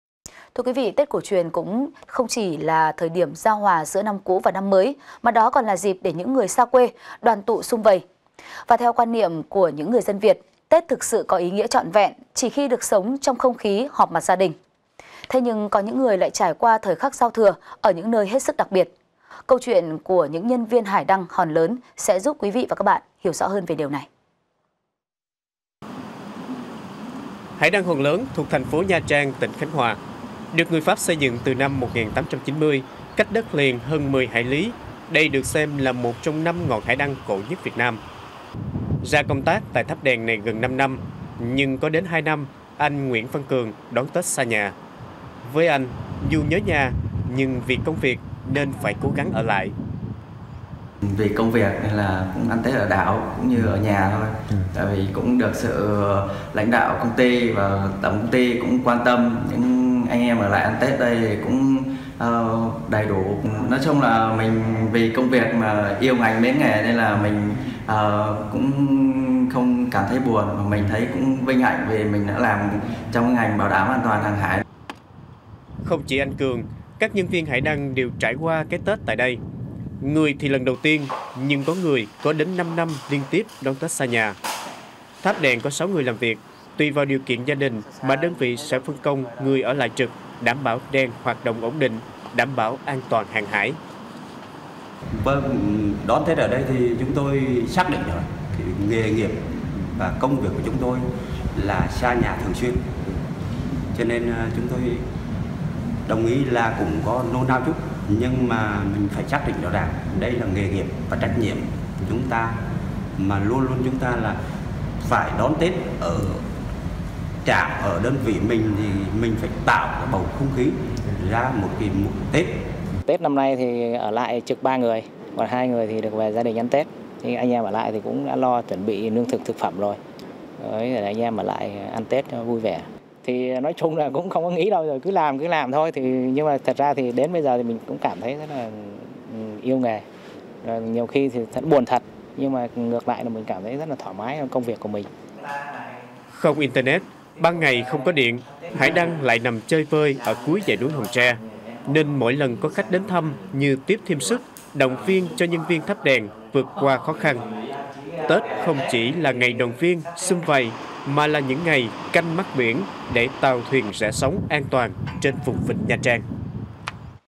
(VTC14)_Đón Tết ở Hải đăng Hòn Lớn
(VTC14) - Tết cổ truyền không chỉ là thời điểm giao hòa giữa năm cũ và năm mới, mà đó còn là dịp để những người xa quê đoàn tựu sum vầy. Và theo quan niệm của người Việt, Tết thực sự có ý nghĩa trọn vẹn chỉ khi được sống trong không khí họp mặt gia đình. Thế nhưng, có những người lại trải qua thời khắc giao thừa ở những nơi hết sức đặc biệt. Câu chuyện của các nhân viên Hải đăng Hòn Lớn sẽ giúp quý vị hiểu hơn về điều đó.